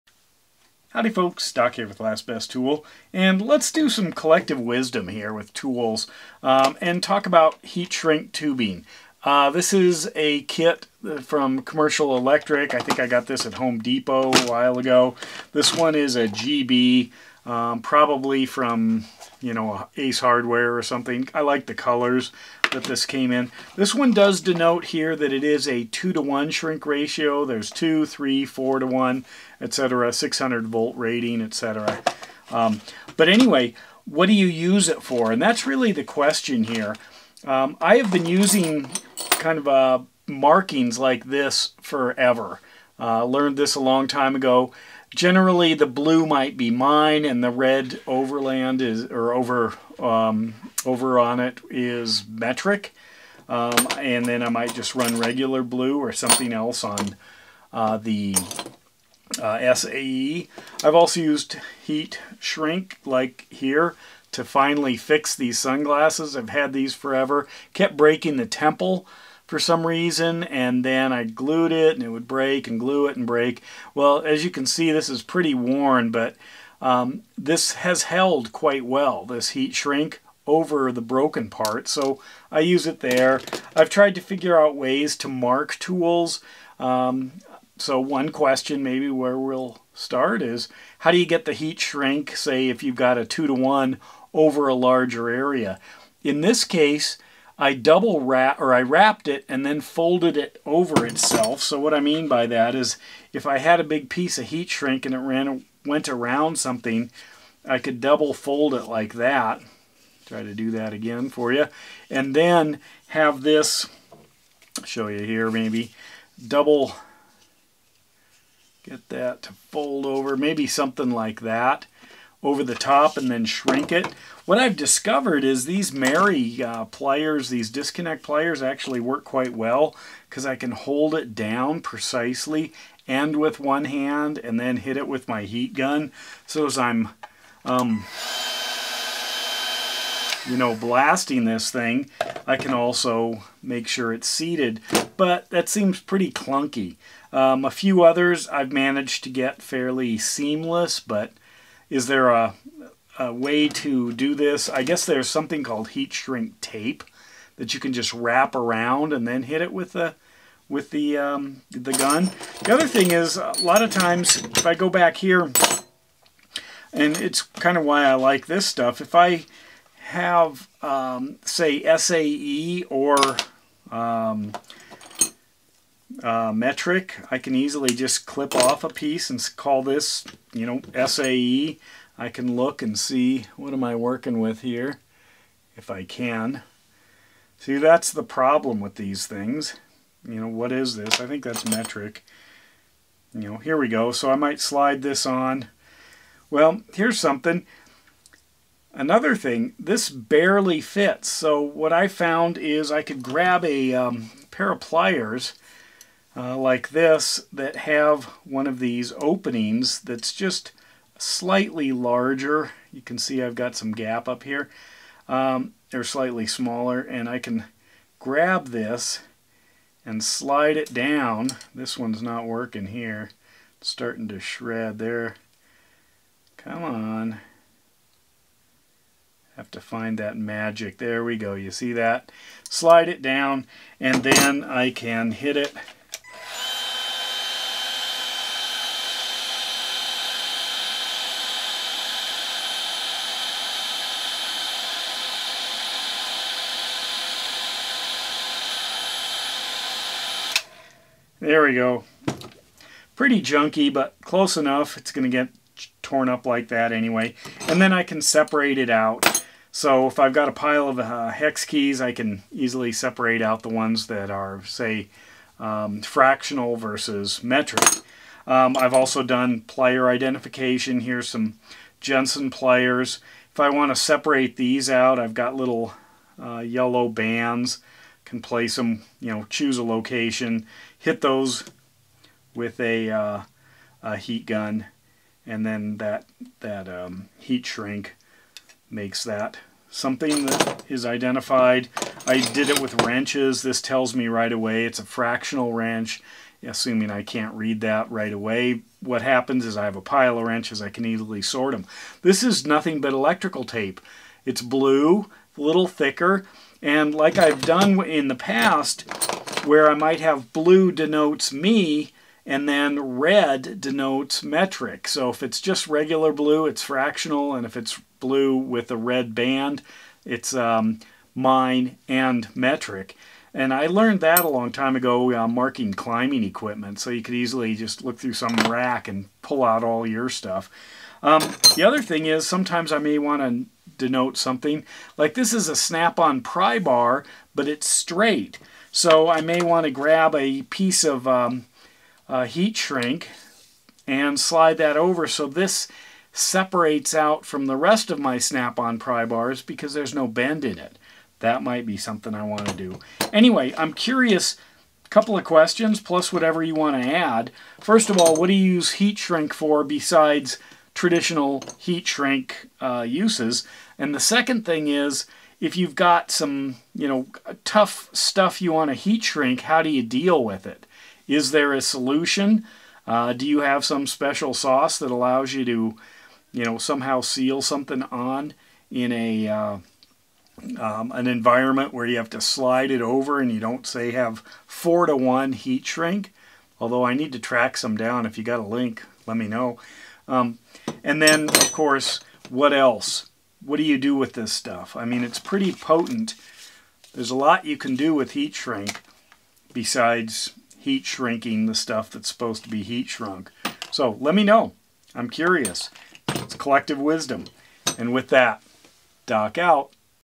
Heat Shrink to the Rescue: Uses, challenges, and solutions with warm plastic. What do you suggest?
Colored Heat Shrink Tubing

Giant colored bundle

4:1 Heat Shrink with adhesive